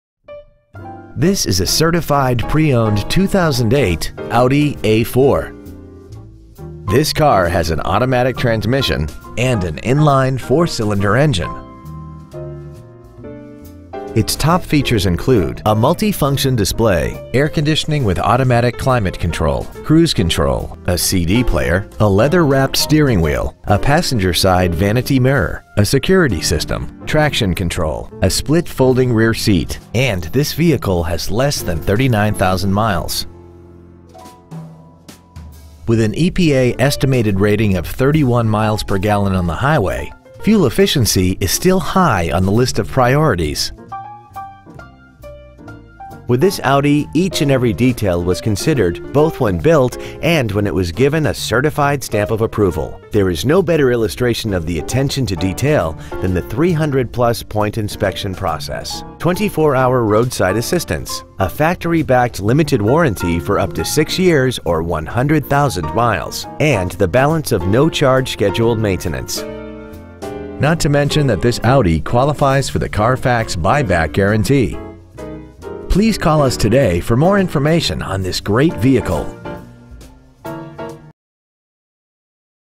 2008 Audi A4 Certified Nashville TN 37204
This is a certified preowned 2008 Audi A4. This car has an automatic transmission, and an inline four cylinder engine. Its top features include a power driver seat, air conditioning with automatic climate control, cruise control, a CD player, a leather wrapped steering wheel, a passenger side vanity mirror, a security system, traction control, a split-folding rear seat, and this vehicle has less than 39,000 miles. With an EPA estimated rating of 31 miles per gallon on the highway, it doesn't compromise fuel efficiency for size, comfort, or fun. With this Audi each and every detail was considered both when built and when it was given a Certified stamp of approval; There is no better illustration of the attention to detail than the 300 plus point inspection process, twenty four hour roadside assistance, a factory backed limited warranty for up to six years or 100,000 miles, and the balance of no-charge scheduled maintenance. Not to mention that this Audi qualifies for the CARFAX buyback guarantee. Stop by today and test drive this automobile for yourself.

 Year: 2008
 Make: Audi
 Model: A4
 Engine: 2.0L I-4 cyl
 Trans.: Automatic
 Exterior: Gray
 Miles: 38,659
 Interior: Black

 Preowned 2008 Audi A4 Nashville TN